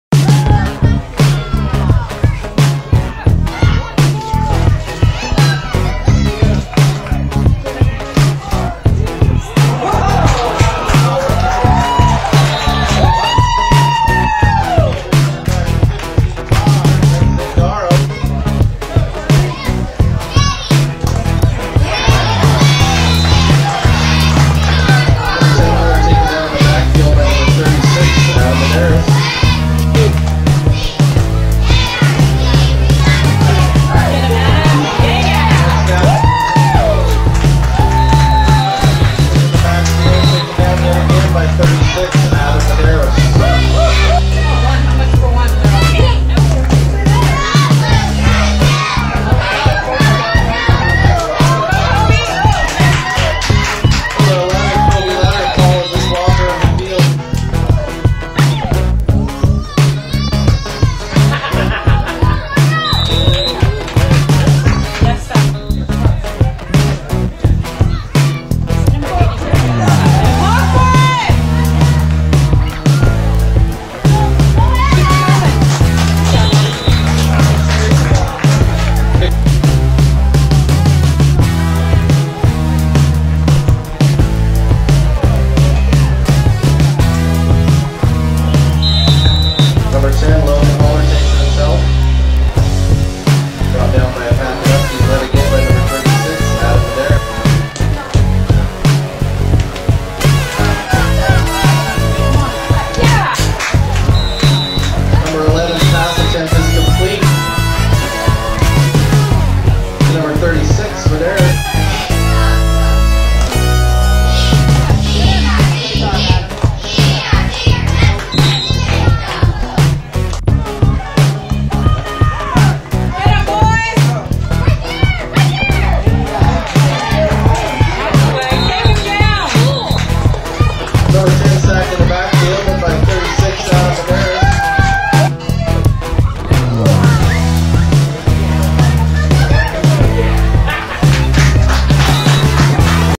Curry Highlights
Adam and teammates had a great game beating Curry 6-0. Adam had some great tackles and some great offensive blocks for his teammates!